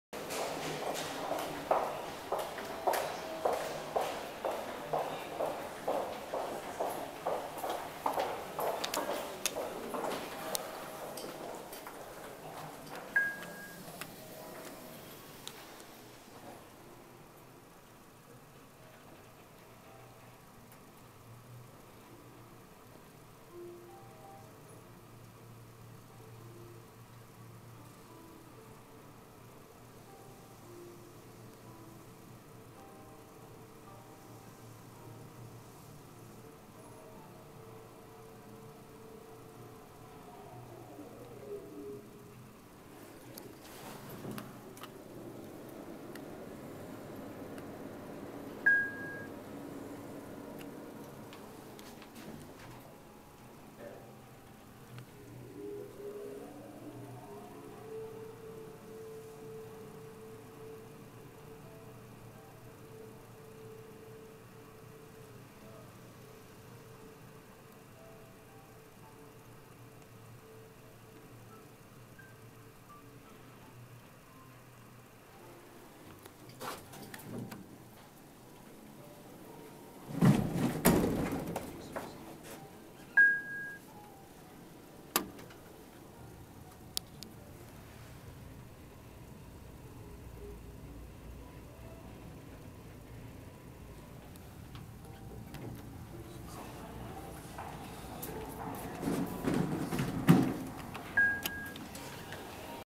パレスサイドビルの西側人荷用エレベーター（17号機）（三菱製）
－－説明－－－－－－－－－－－－－－－－－－－－
毎日新聞社の本社がある「パレスサイドビル」の17号機(西側)人荷用エレベーターです。
更新済みで、普段は業務メインとして使用されます。
開閉ボタンのデザインがエレアド仕様です。
－－諸元－－－－－－－－－－－－－－－－－－－－
メーカー:三菱
機種:アクセルアイ
用途:人荷用
定員:20名
積載:1500kg
アナウンス:×
到着チャイム:○
速度：90m/min
駆動方式:ロープ
停止階:B5,B4,B3,B2,B1,1,2,3,4,5,6,7,8,9,MR,R
設置数:1基
車椅子操作盤:×
防犯カメラ:○
メンテナンス:三菱電機ビルテクノサービス
設置年:1966年→1990年代後半更新

西側乗用2（詳細不明）→未撮影
西側人荷用(17号機)→Here
－－設置場所－－－－－－－－－－－－－－－－－－
場所:東京都千代田区一ツ橋1丁目1-1